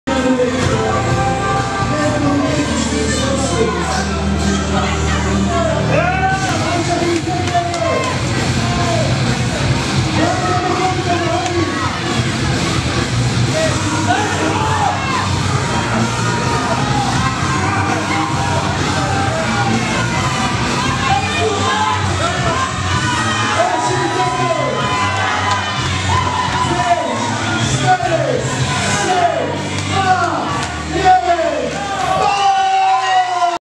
SPORTS TS WINTER TEAM BATTLE VOL. 2
27 stycznia odbyły się w Siedlcach zawody crossfitowe SPORTS TS WINTER TEAM BATTLE VOL. 2